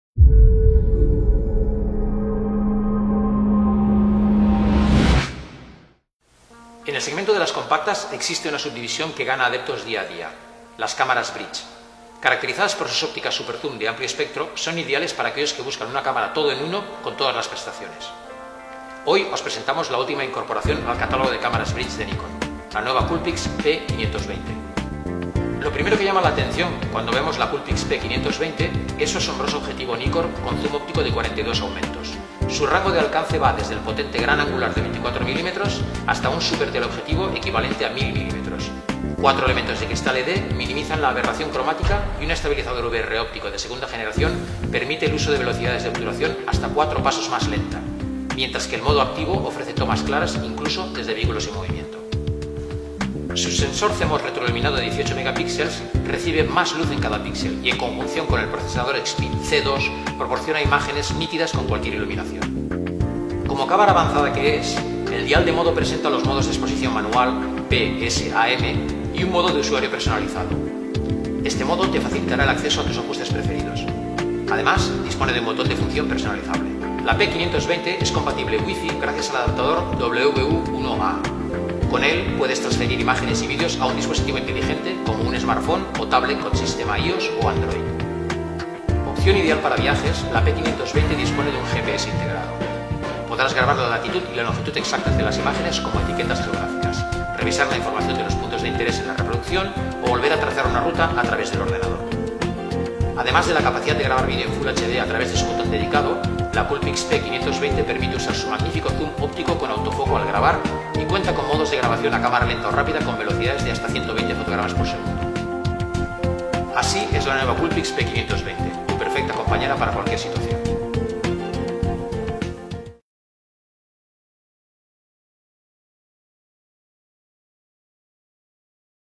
Nikonistas TV: Coolpix P520
Dé rienda suelta a su creatividad con la COOLPIX P520, la cámara bridge con un súper zoom de 42 aumentos. Con un objetivo NIKKOR que ofrece una increíble cobertura de 24-1000 mm desde gran angular hasta súper teleobjetivo, esta cámara de 18 megapíxeles se acerca a la acción todo lo que desee. El sensor CMOS retroiluminado de 18 megapíxeles y la reducción de la vibración de desplazamiento de lente avanzada habilitan la estabilidad de las tomas con cualquier iluminación, mientras que la pantalla LCD abatible ofrece una vista perfecta de alta resolución desde cualquier posición. El modo de vídeo HD permite cambiar a la filmación de vídeos de manera instantánea con un solo toque y el GPS integrado incluye "etiquetas geográficas" en los datos de la imagen.